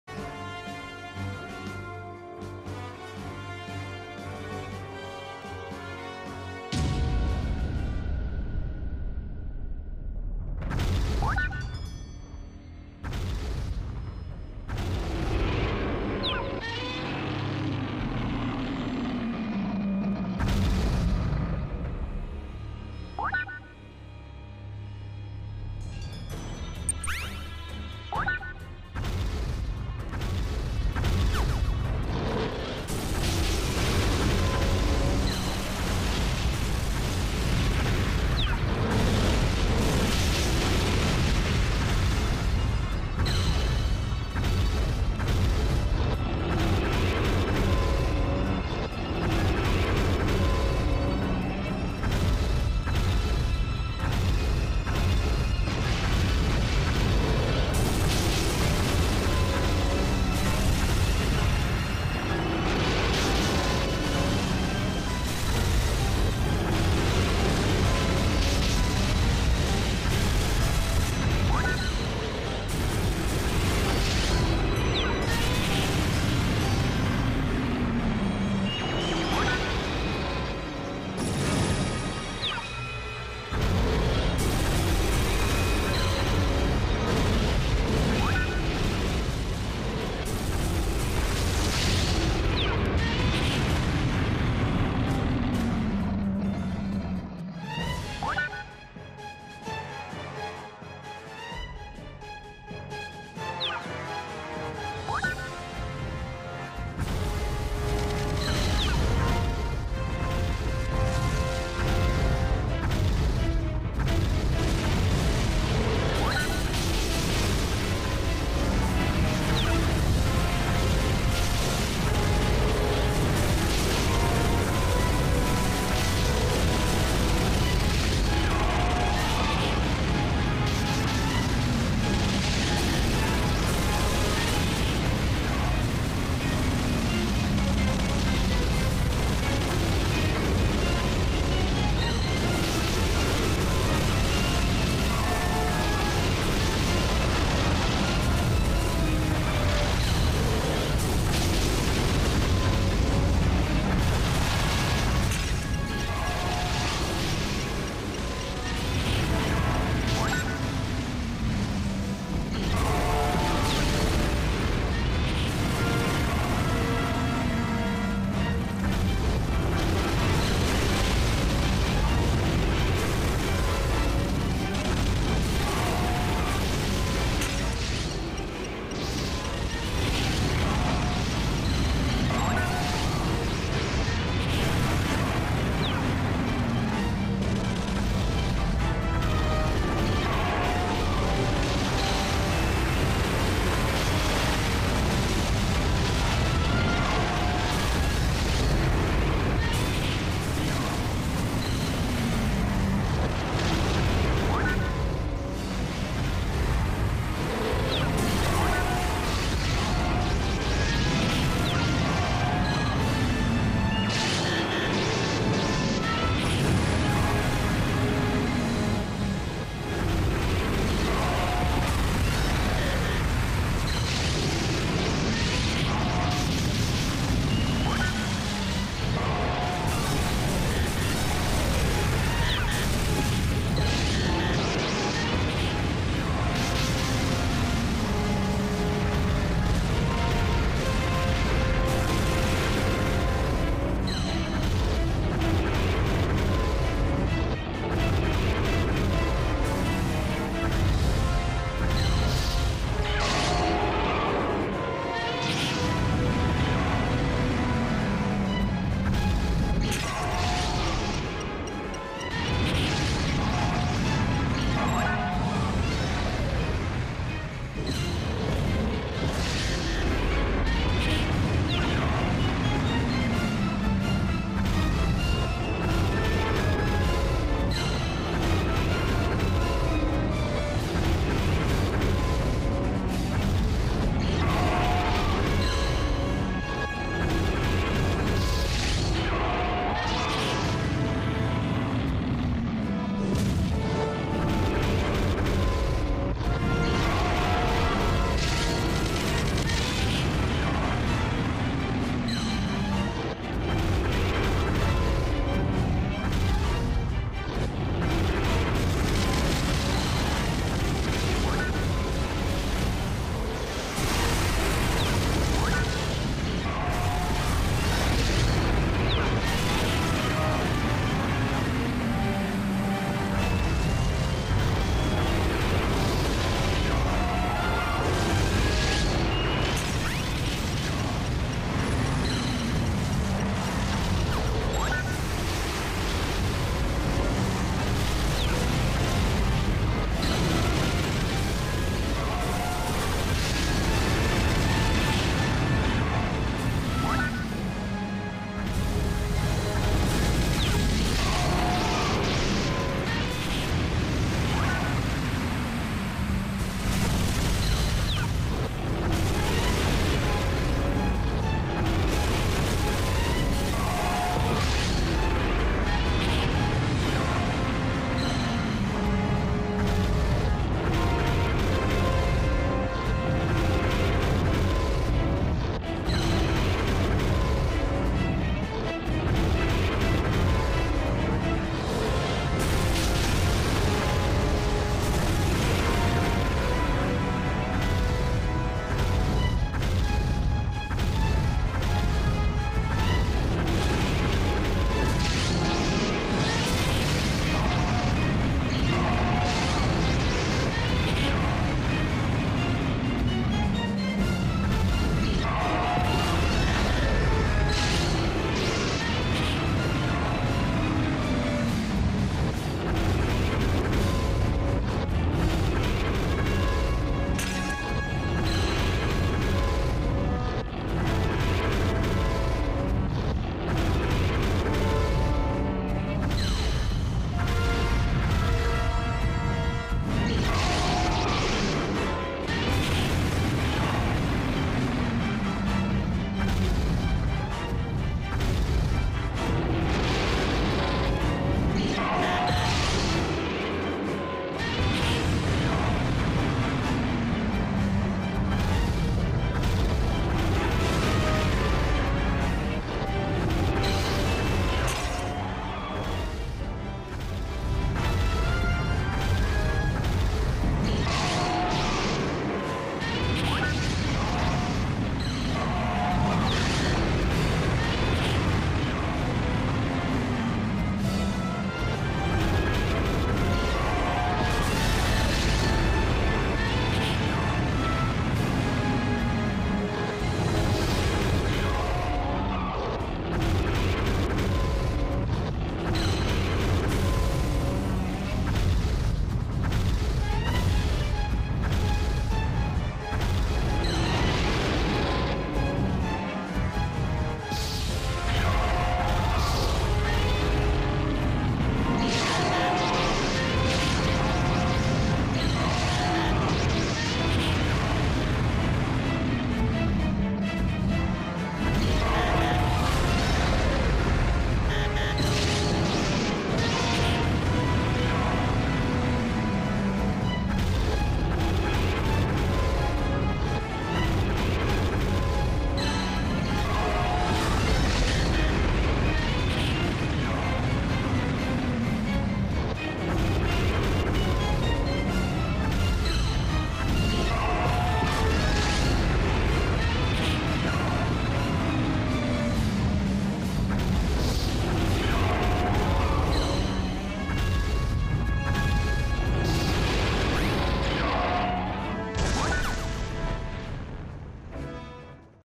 Bigass Battra must die!
Round 3 of bigass kaiju vs tiny Godzilla.